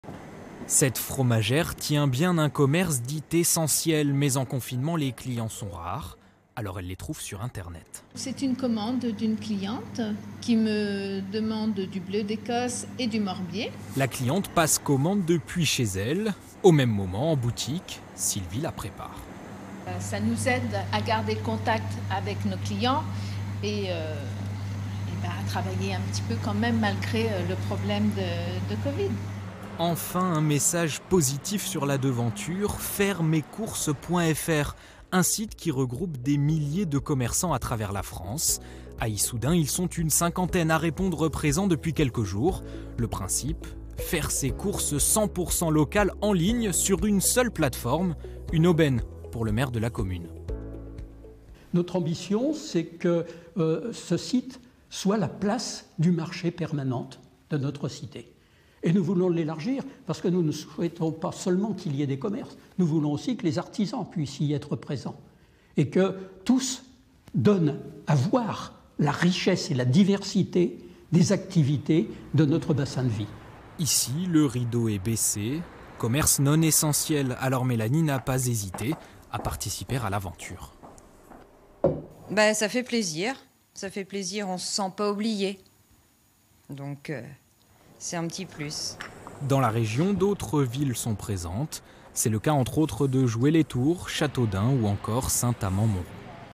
Issoudun : possibilité de faire ses courses en ligne et 100% local avec fairemescourses.fr
Depuis plusieurs semaines on entend beaucoup parler du click-and-collect en français : la vente à emporter. Le principe est simple plusieurs  commerçants d'une commune sont réunis sur une seule et même plateforme en ligne.

C'est le cas de plusieurs boutiques à Issoudun dans l'Indre.
Reportage :  Antoine Wernert et Marion Ptak.